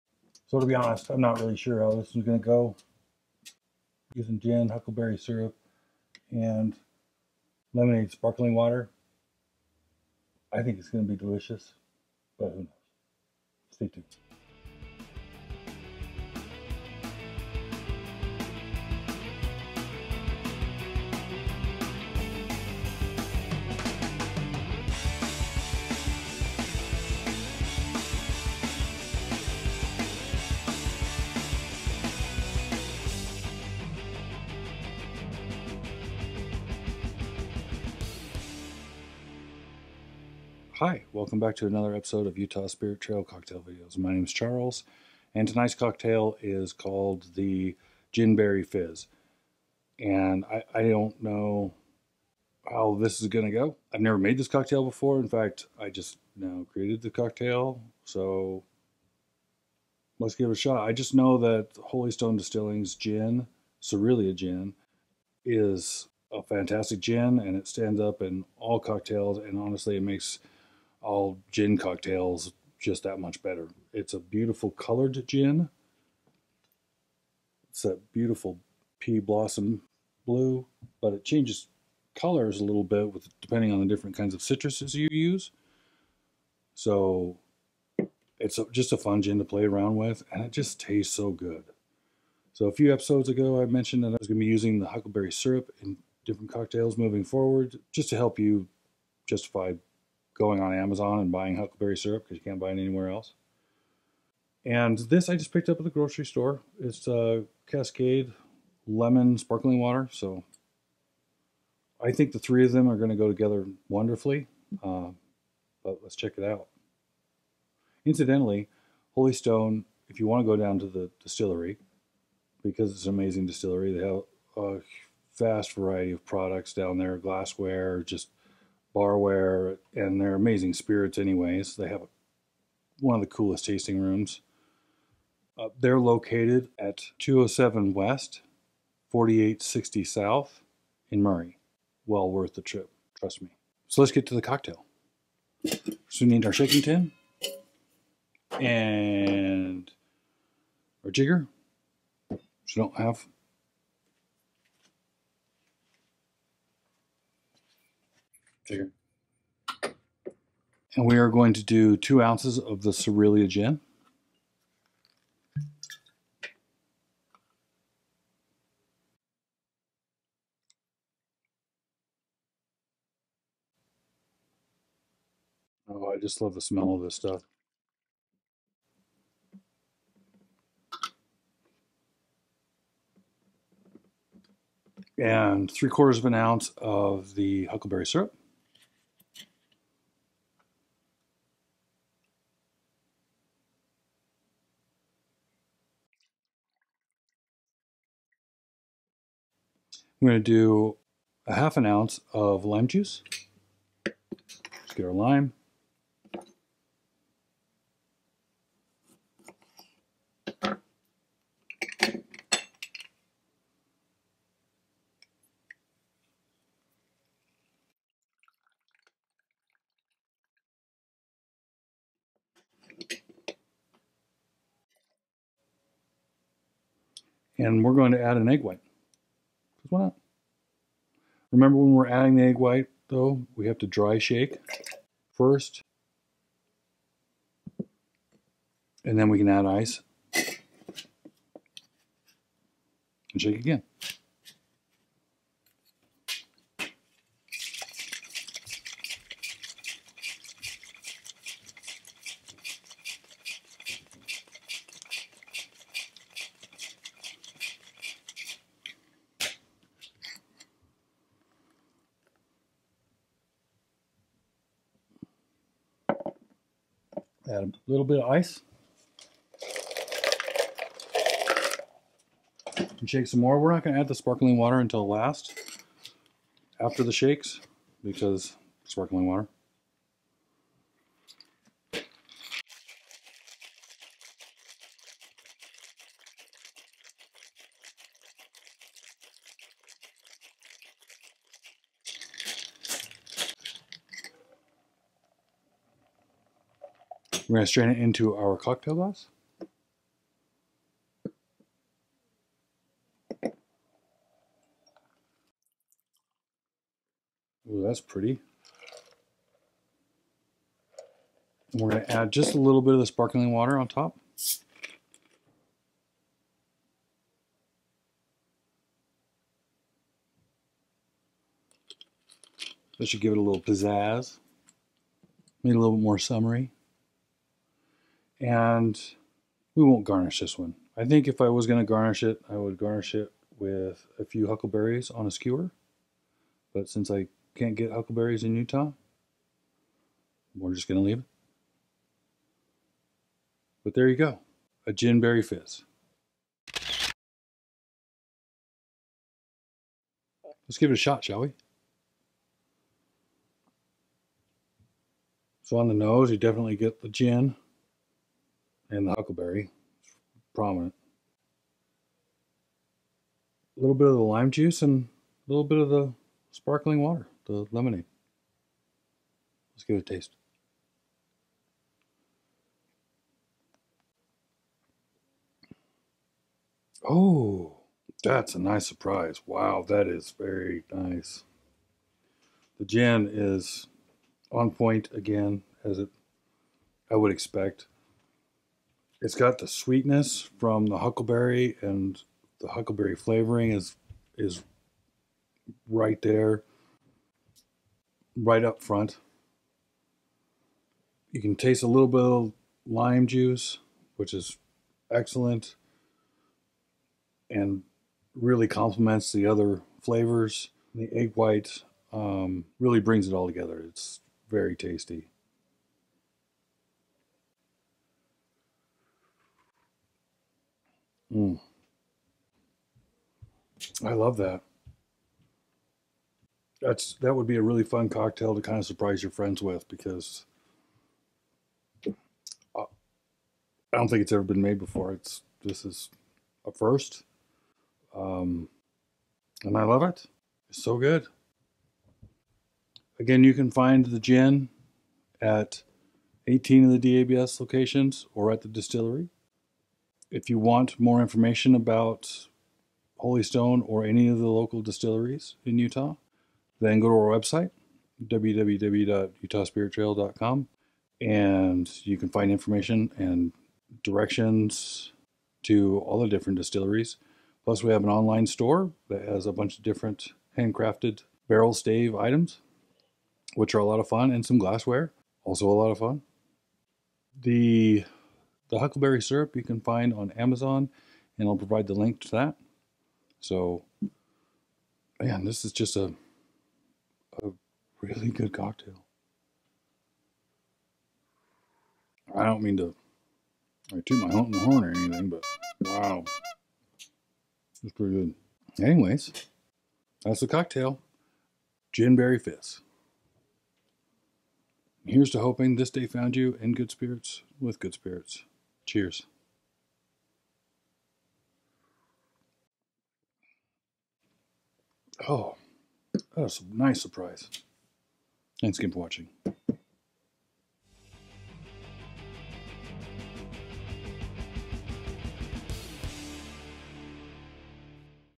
Spirit Trail Cocktail Videos Ep.
The Gin-Berry Fizz features one of the distilleries on Utah's Spirit Trail, Holystone Distilling. As is the case with all Spirit Trail Cocktail Videos this cocktail is inspired by the men and women who craft their spirits right here in Utah.

And Twitter: @Utahspirittrail

Gin-Berry Fizz:
2 oz / 60 ml Cerulea Gin
.75 oz / 22.5 ml Huckleberry Syrup
.5 oz / 15 ml Lime Juice
1 Egg White

Add gin, huckleberry syrup, lime juice, and egg white to shake and tan and shake for 5 to 10 seconds. Then add ice and shake for another 10 to 15 seconds. Strain into a cocktail glass and top with lemonade sparkling water. If you have access to huckleberries, skewer 3 and add them on top of the glass.